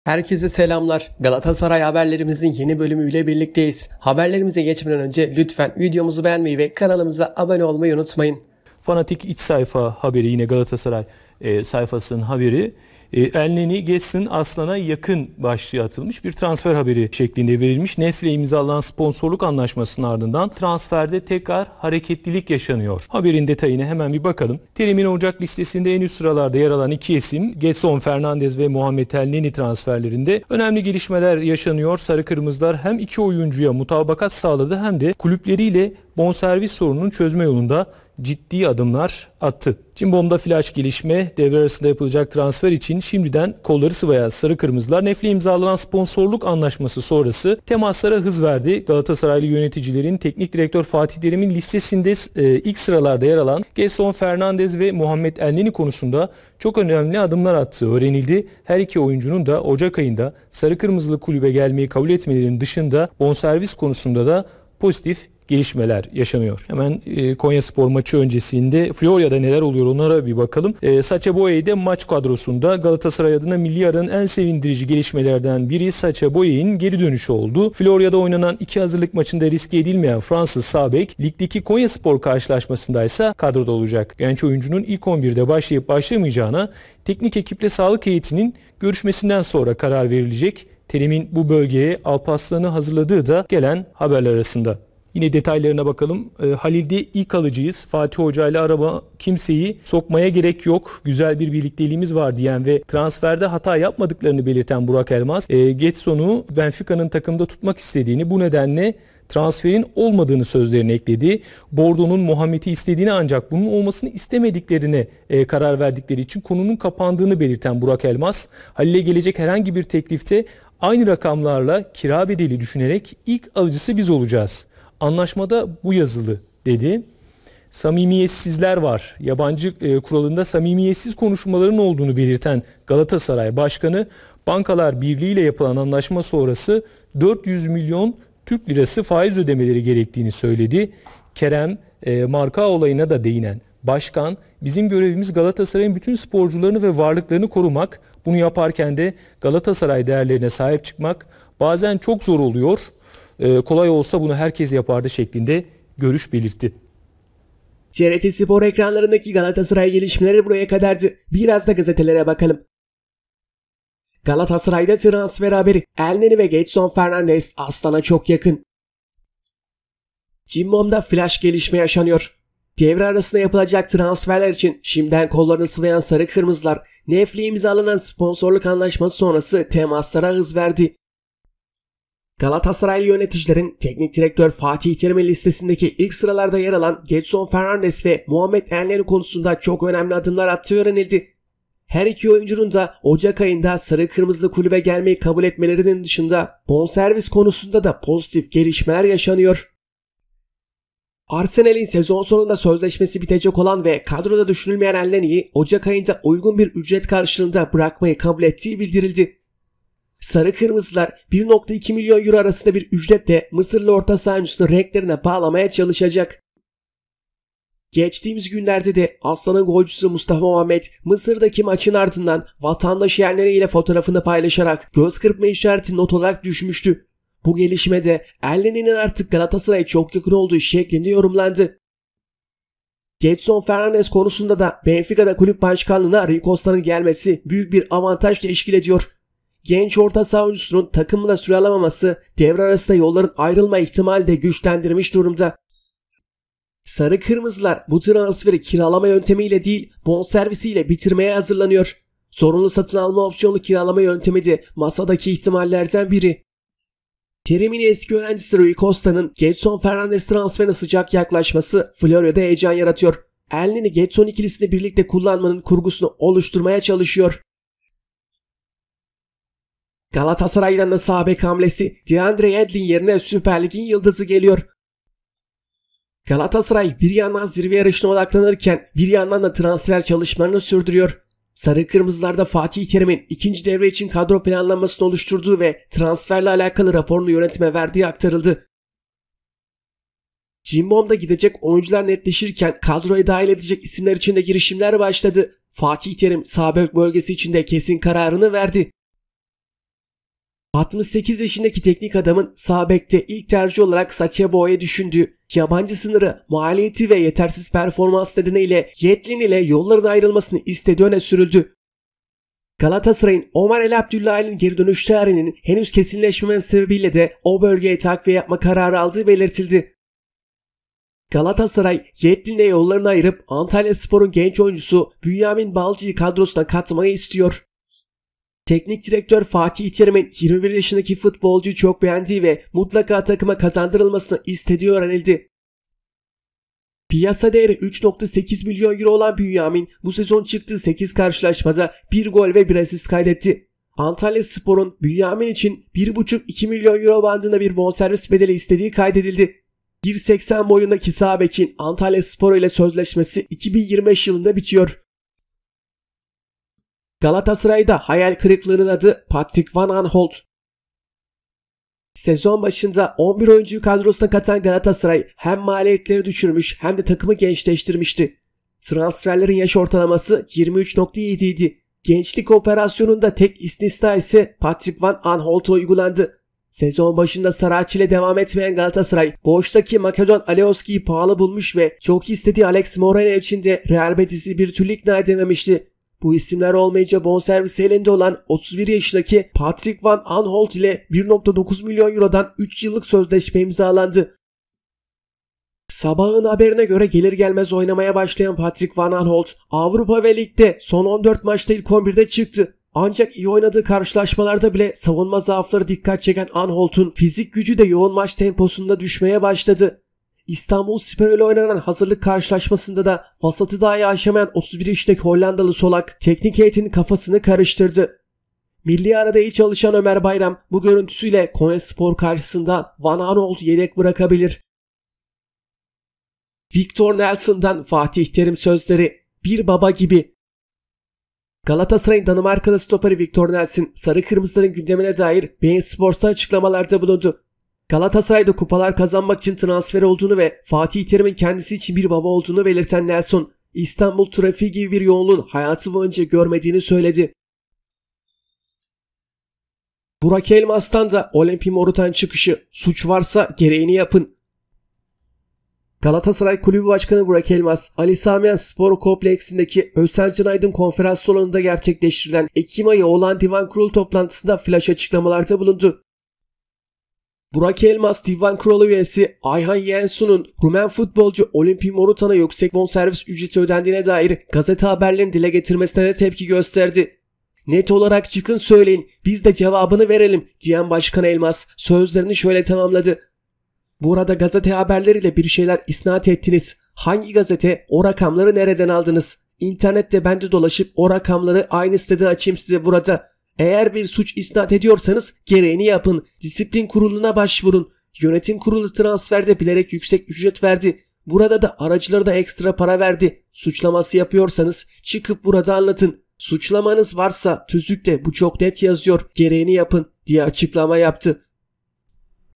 Başkan Göz Kırptı !! Gedson & Elneny Transferi !! Sağ Beke Bünyamin Balcı !! l GALATASARAY
Galatasaray'da Ocak ayı transfer dönemi hareketli geçecek.. İlk tercih Gedson Fernandes ve Elneny transferi..

İyi seyirler..